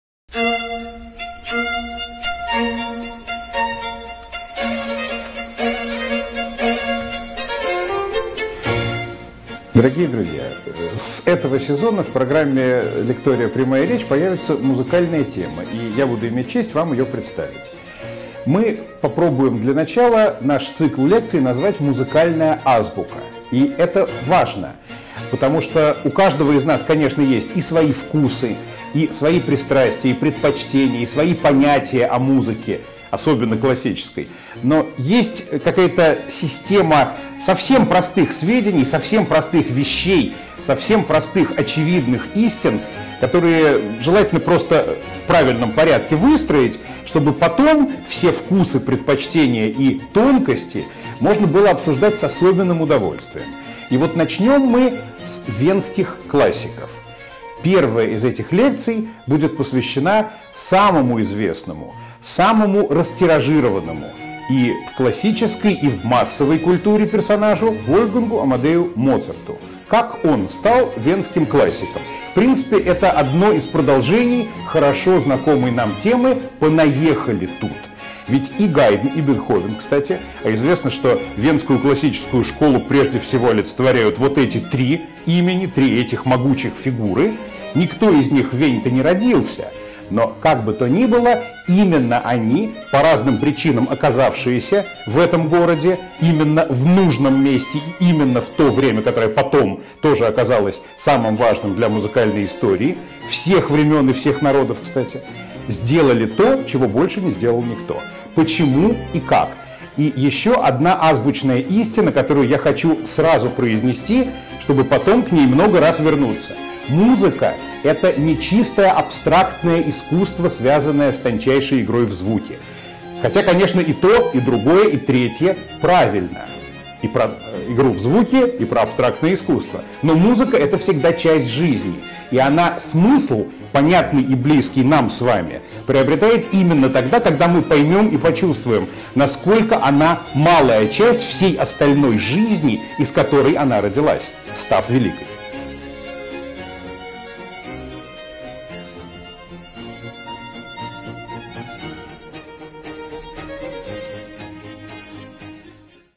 Артем Варгафтик анонс лекции "Венские классики. Моцарт"
Лекция будет посвящена самому известному, самому растиражированному (и в классической и в массовой культуре персонажу) -- Вольфгангу Амадею Моцарту. Как он стал венским классиком?
Это одно из продолжений хорошо знакомой нам темы: «понаехали тут». Ведь ни Гайдн, ни Бетховен, ни Моцарт (а венскую классическую школу олицетворяют в первую очередь три этих имени, три эти могучие фигуры) - никто их них в Вене не родился. Но именно они, по разным причинам оказавшись в этом городе в нужное время -- самое важное для музыкальной истории всех времен и всех народов, они сделали то, чего не сделал больше никто. Почему и как?

Лекция состоится в ЕКЦ на Никитской /Б Никитская, 47/

организатор лекторий "Прямая речь"
билеты онлайн на сайте pryamaya.ru
и в кассах ЕКЦ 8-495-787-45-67